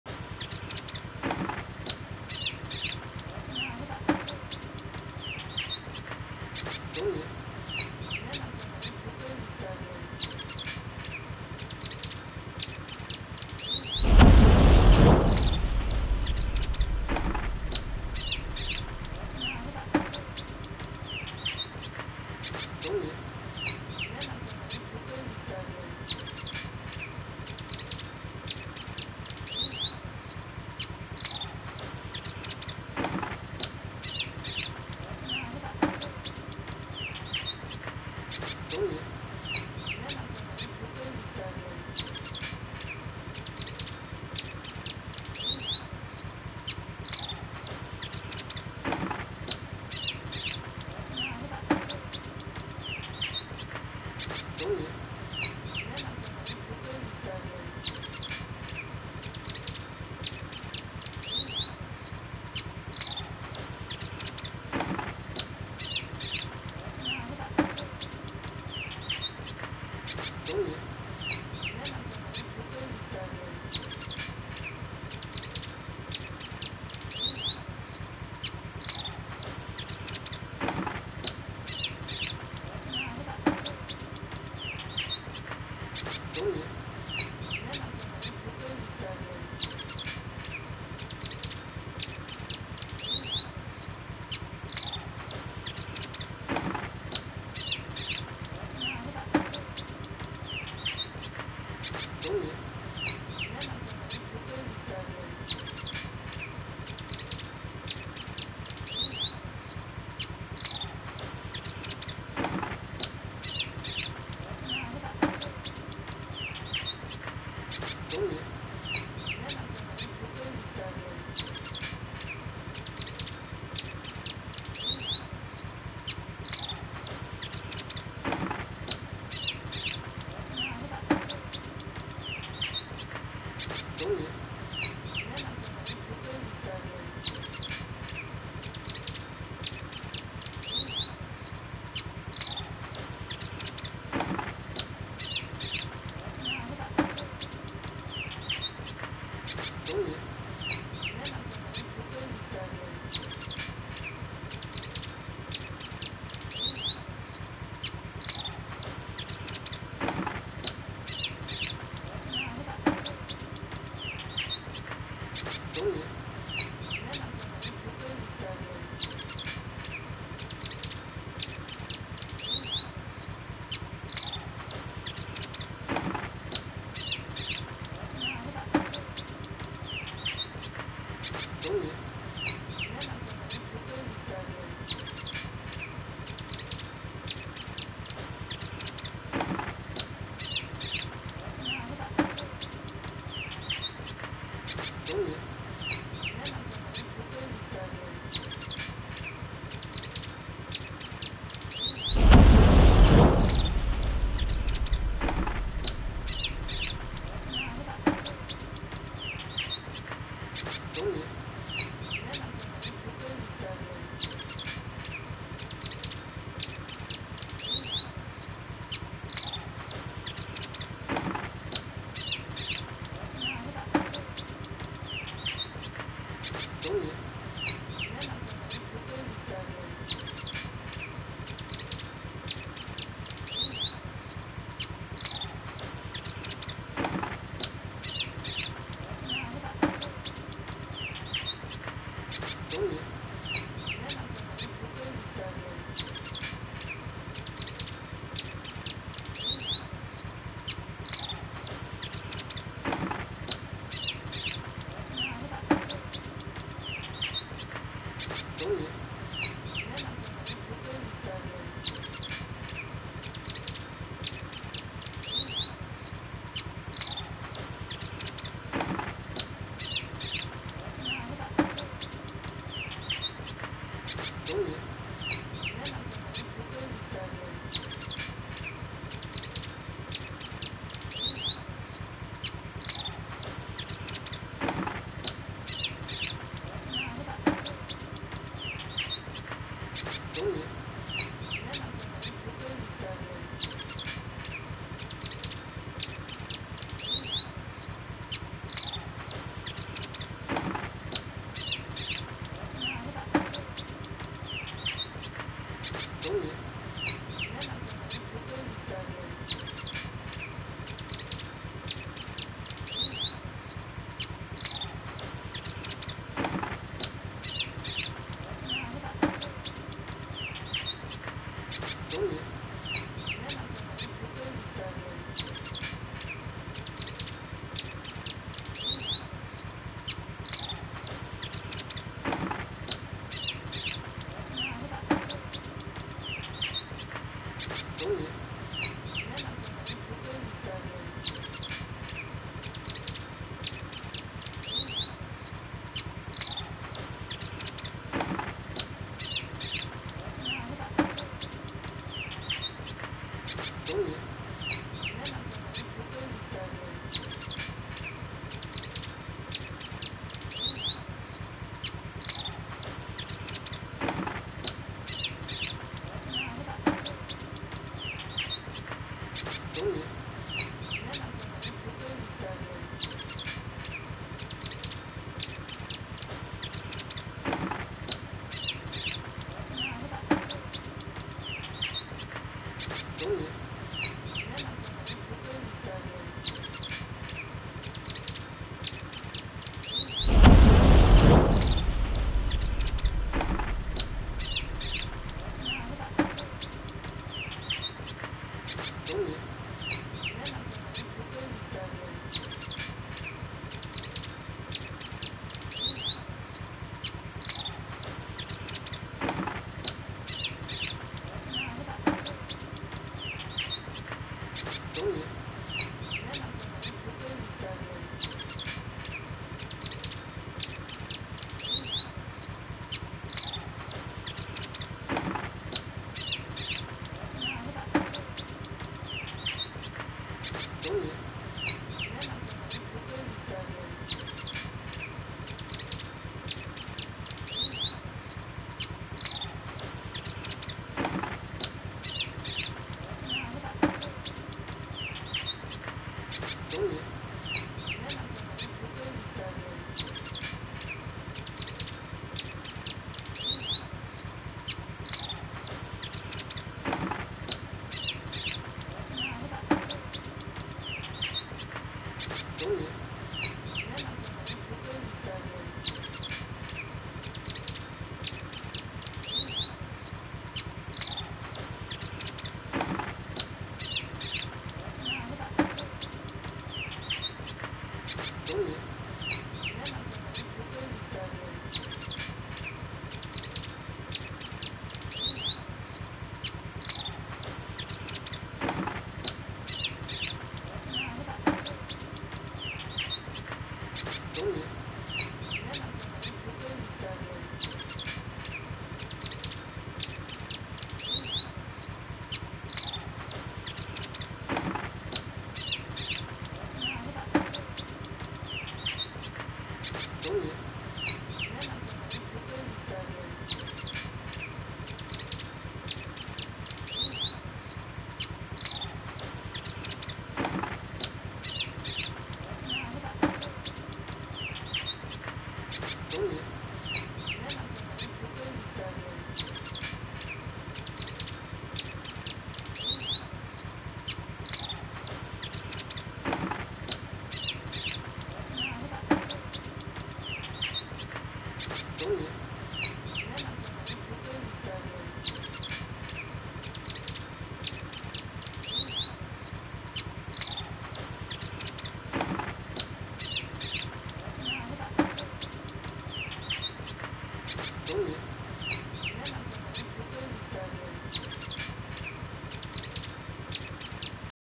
pancingan tercepat ampuh bikin konin bahan & ombyokan ikut bunyi ||| mater kacer betina
pancingan tercepat ampuh bikin konin bahan & ombyokan ikut bunyi

Kacer betina memanggil kacer jantan